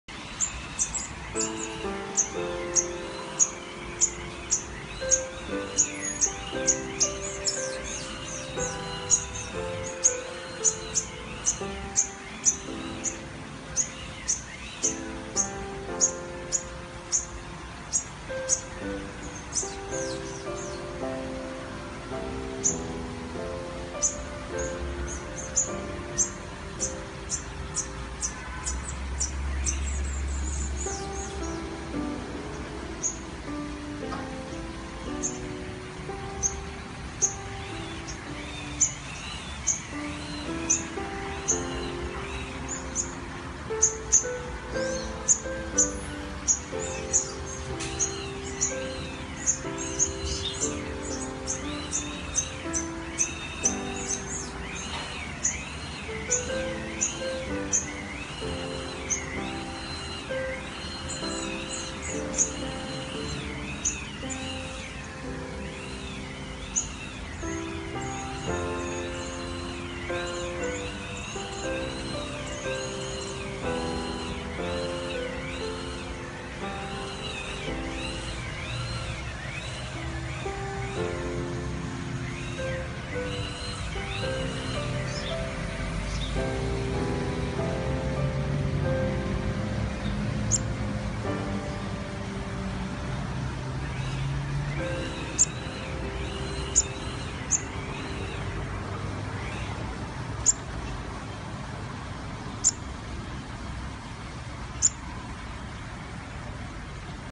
Mama sunbird busy with 2 babies
Is mama sunbird coaxing the baby bird to fly with her?